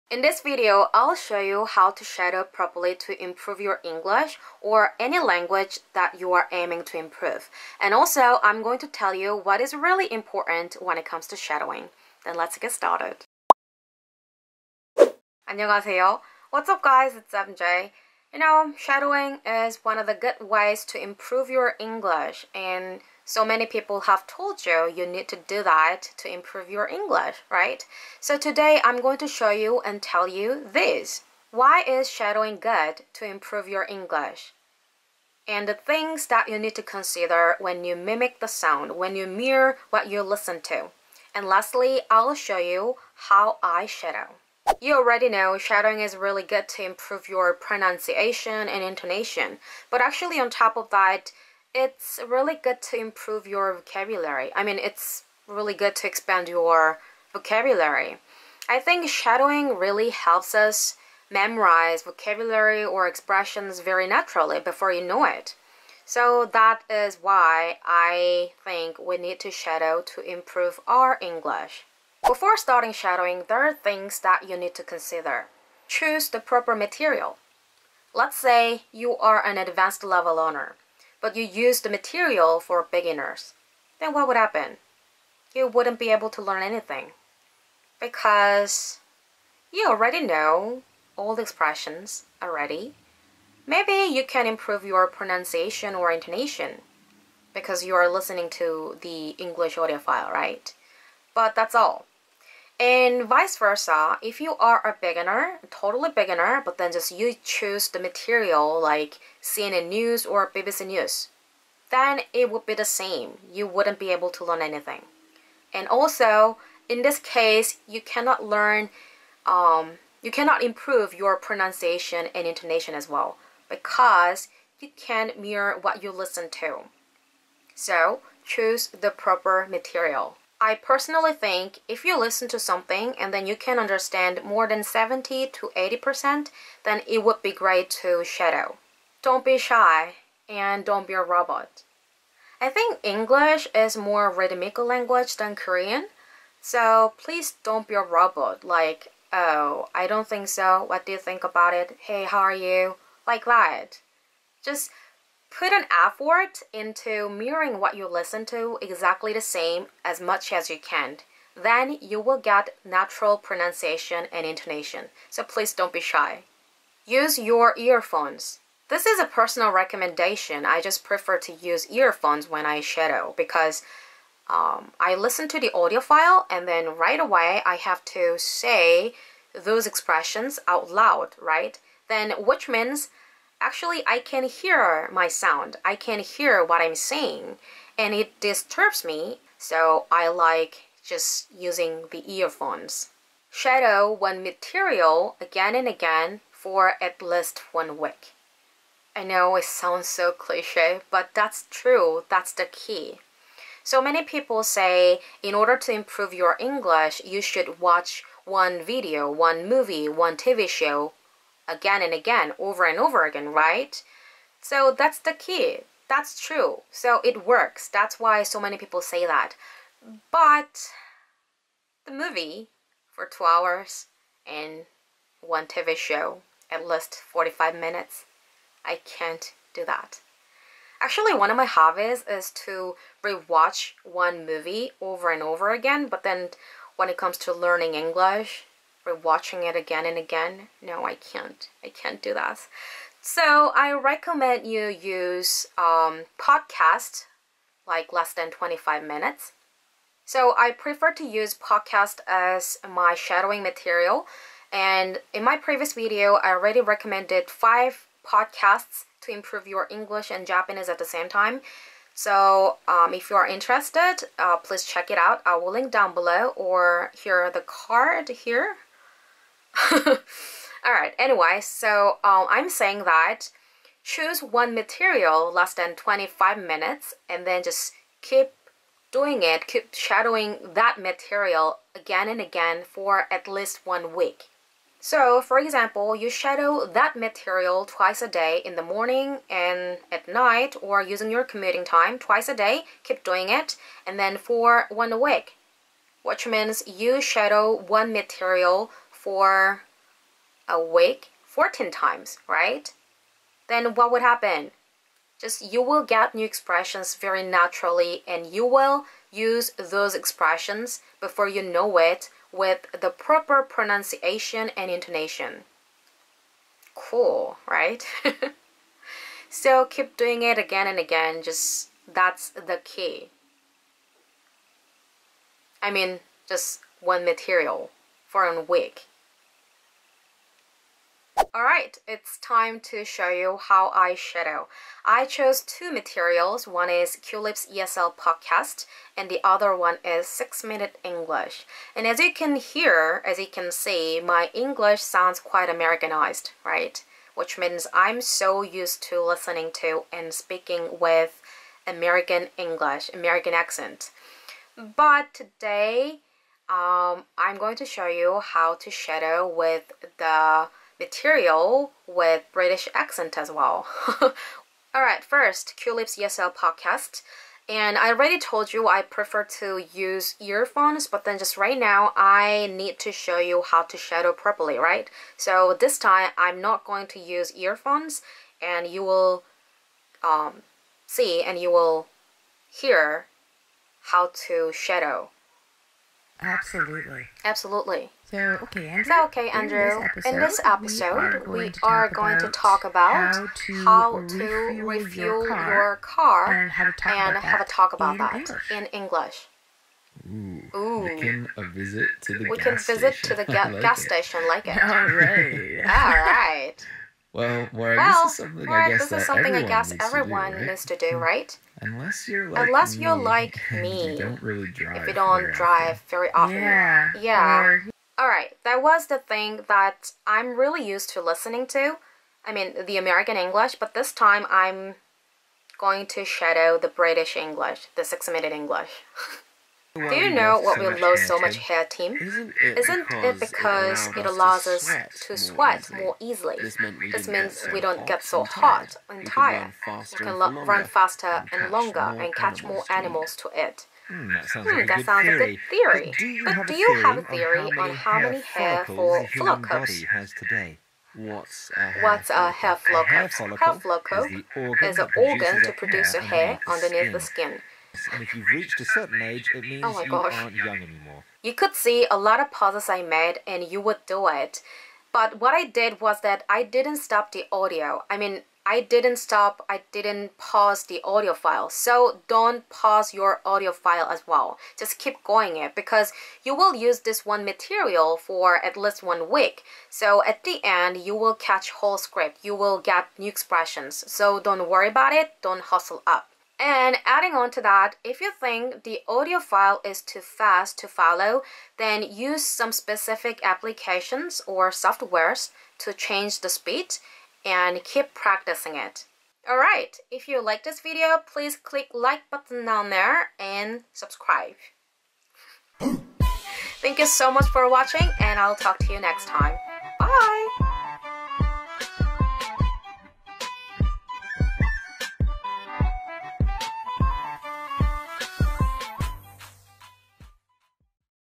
영어 쉐도잉 방법 101 & 영어 쉐도잉 효과 😉👏 | 영어 영상
영어 쉐도잉(섀도잉) 방법과 쉐도잉(섀도잉) 효과, 유의 사항이 궁금하신가요? 모두 털어 드립니다! 영어 쉐도잉이 스피킹 실력 늘리기에 좋은 방법이라는 걸 아실 거예요. 발음, 억양 교정뿐 아니라 표현 암기에도 효과가 있다는 거 아시나요? 귀 쫑긋 열고 함께 꼬고고! ! ❤️

I'm going to tell you how to improve your English speaking skills, which is Shadowing.
My English has been improved with this method.
And you can improve it by yourself!!
I'll show you how I shadow English materials.